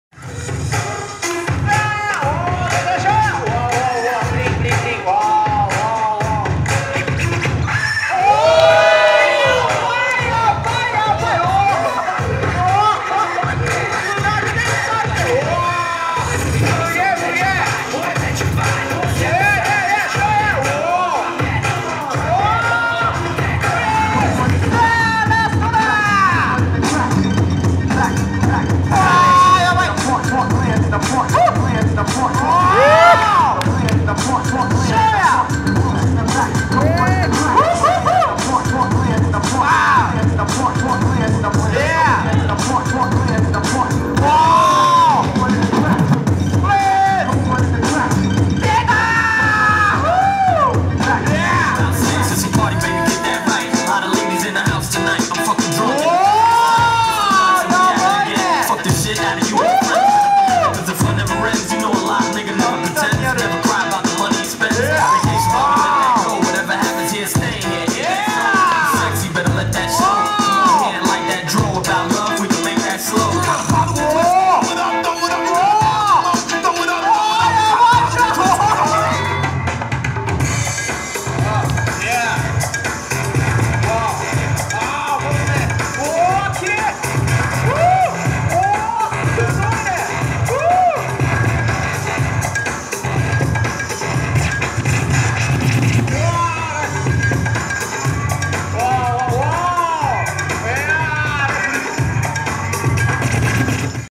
Bboy Shoya driving the MC crazy at Dare 2 Differ 2019. Who's Got Daily Flava #2
When you're killing it so much that the MC starts screaming! Bboy Shoya's (Mortal Combat) blow ups and power is enough to make anyone start going crazy...